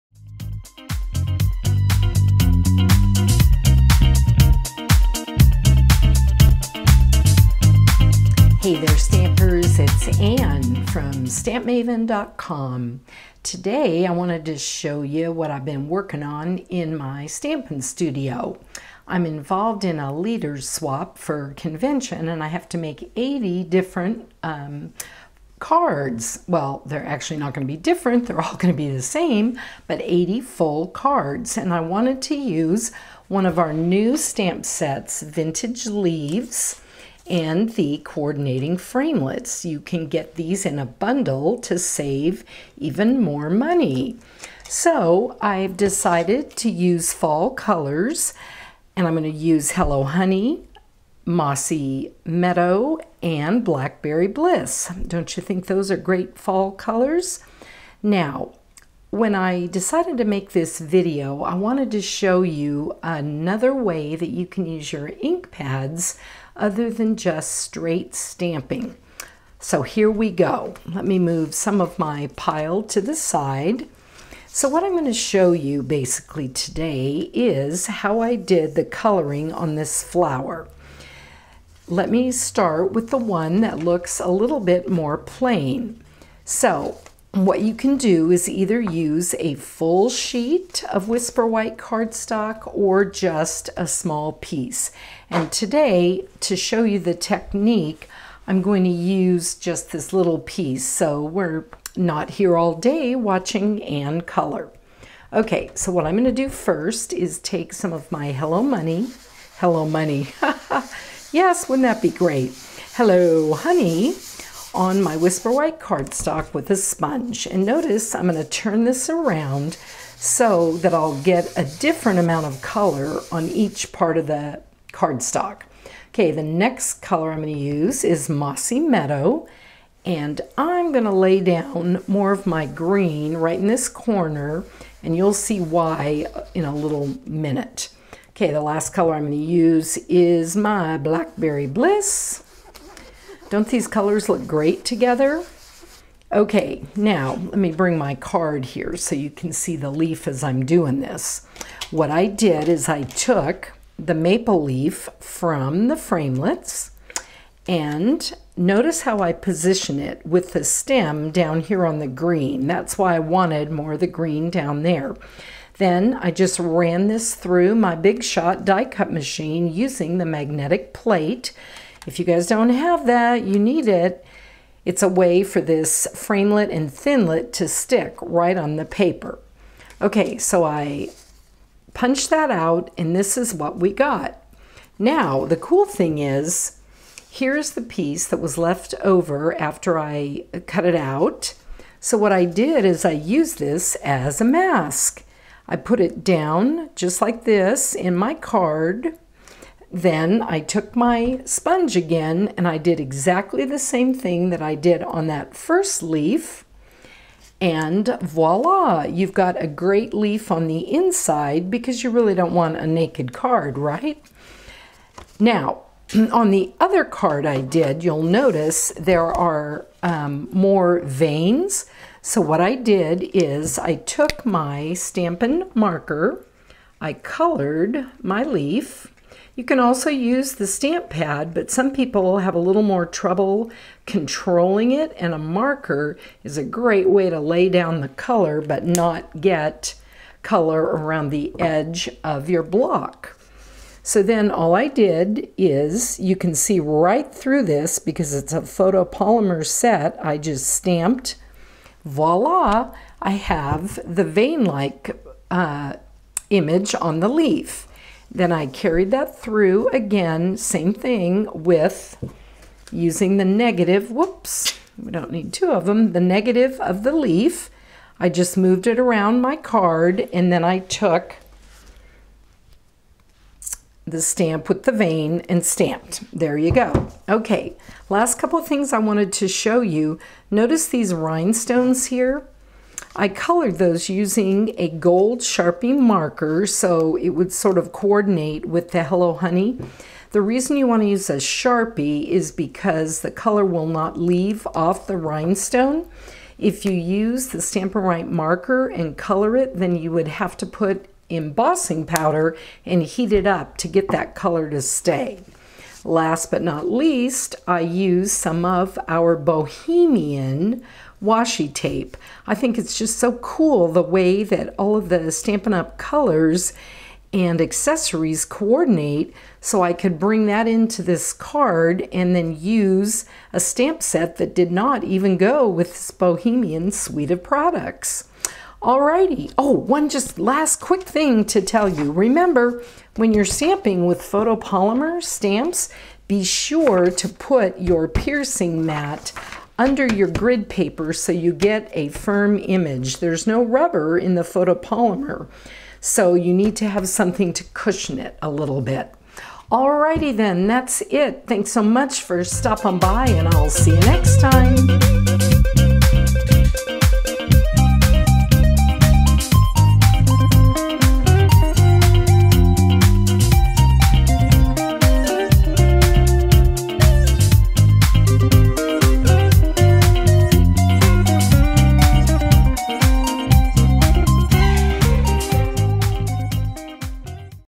Inked Sponge Coloring
In this video, Anne shows you how you can use your stampin' sponges to create interesting effects. The Inked Sponge Coloring Technique is quick and easy and you'll be amazed at what you can do with it.